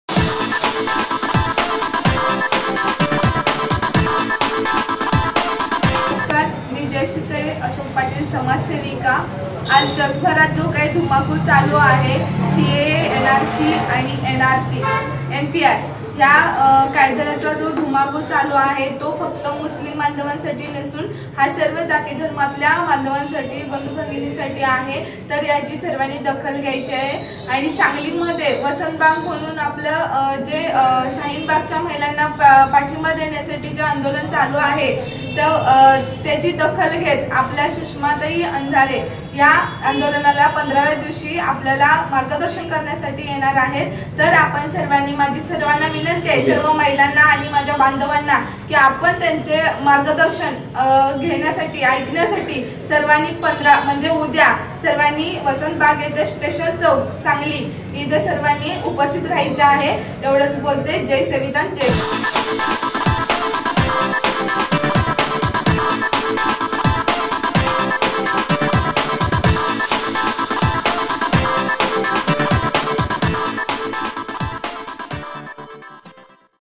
Youth#leader
सांगलीच्या वसंतबाग आंदोलनात उद्या सुषमा अंधारे यांची तोफ धडधडणार